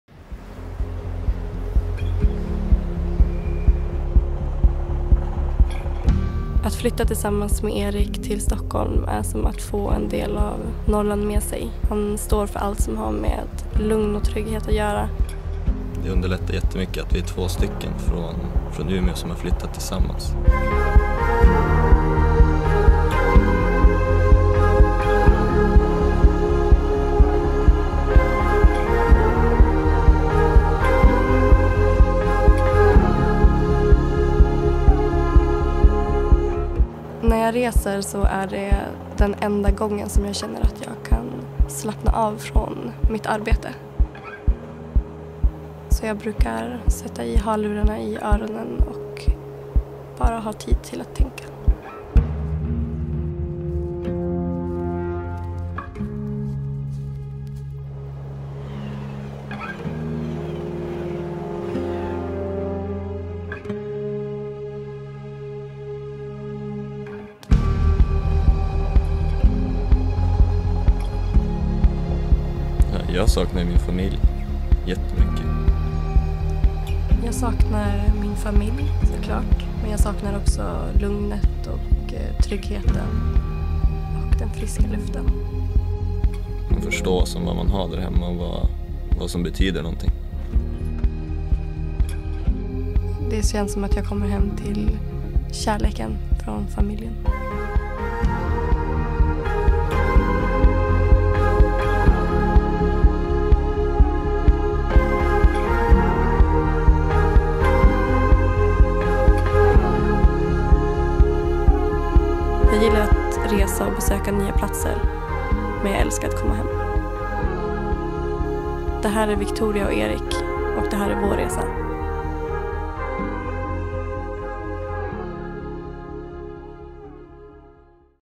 Resan hem med Victoria Törnegren & Erik Forsgren
Alla gör vi olika resor i livet.  En av dem är resan hem. Victoria Törnegren sätter Sverige på mode-kartan med sin minimalistiska stil som hon alltid sätter sin egna prägel på. Hennes sociala kanaler frontas inte bara av mode utan även av resor och livsstil. Detta ofta tillsammans med pojkvännen och modellen Erik Forsgren som även han är en influencer som tagit stor mark på kort tid. Erik är även han uppmärksammad för sin känsla för mode, han har bland annat hamnat på tidningen Cafés topplista över Sveriges 50 Bäst Klädda Män under 2016. De båda kommer från Umeå men bor idag i Stockholm. Det här är deras resa hem.